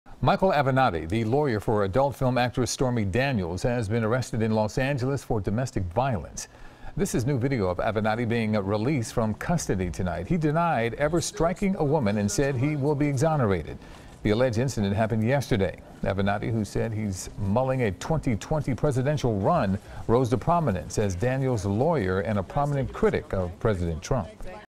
Michael Avenatti Released From Custody Following Arrest For Alleged Domestic Violence
Avenatti, the lawyer for adult film star Stormy Daniels, denies ever hitting a woman.